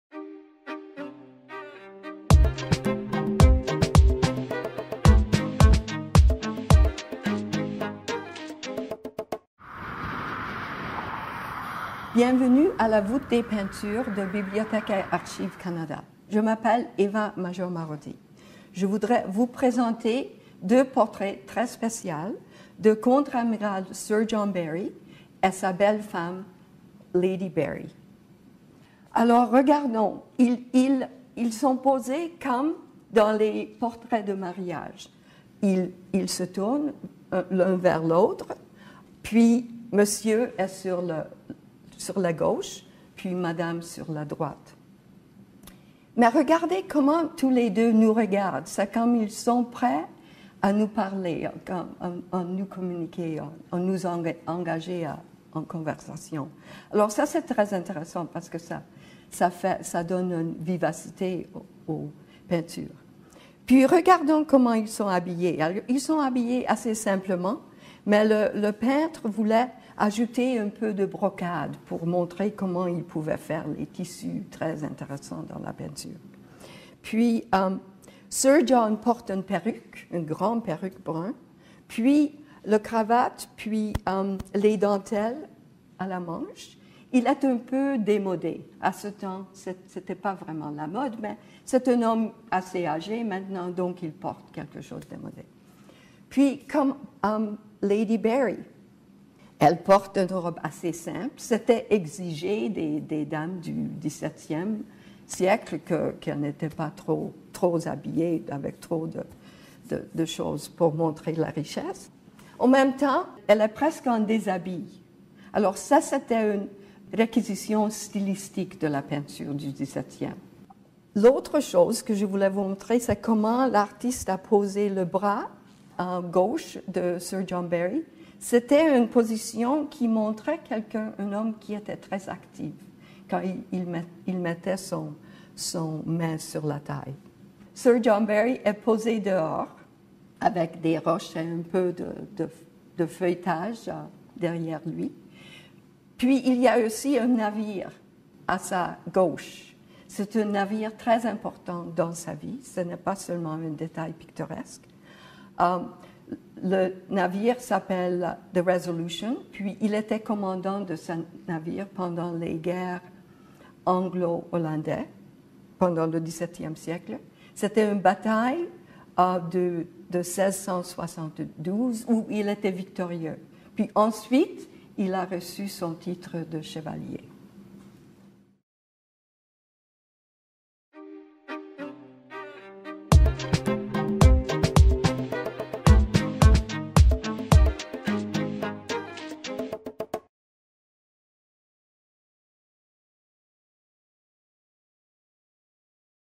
Lord et Lady Berry
Les tableaux, les plus anciens portraits de la collection de Bibliothèque et Archives Canada, représentent un des pères fondateurs de Terre-Neuve et son épouse.